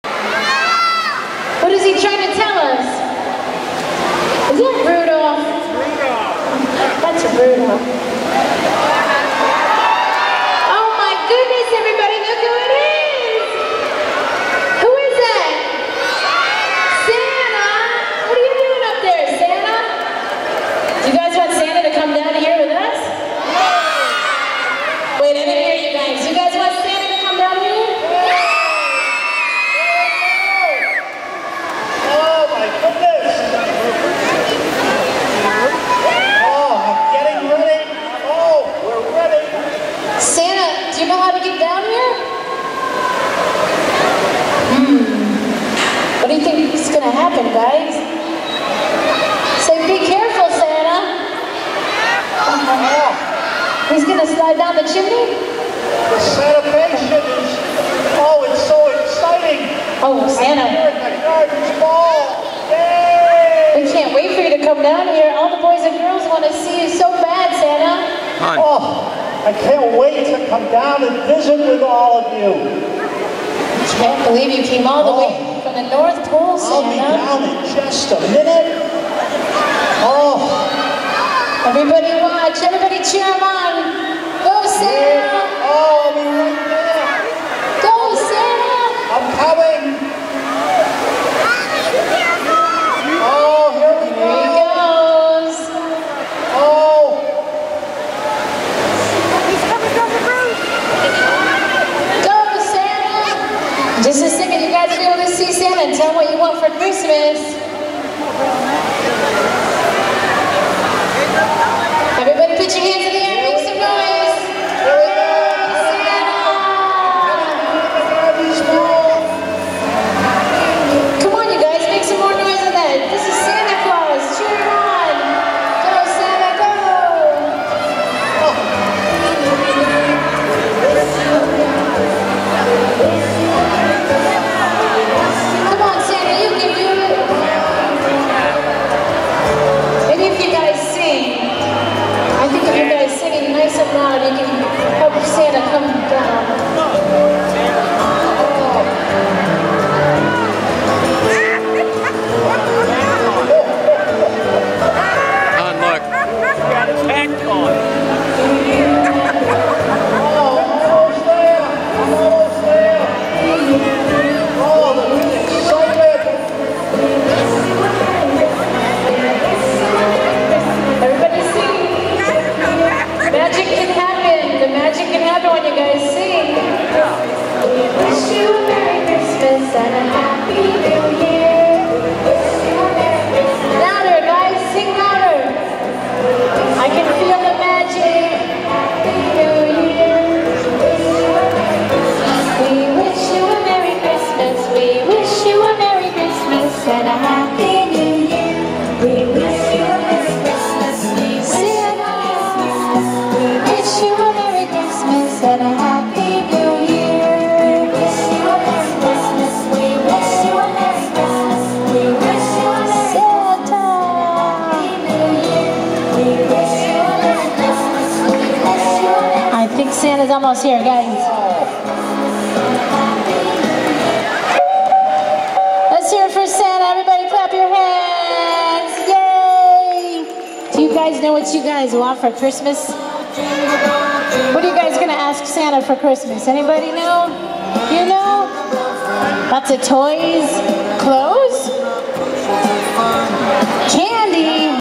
Santa Fail - The Repel
Santa has a minor incident while trying to repel from the roof..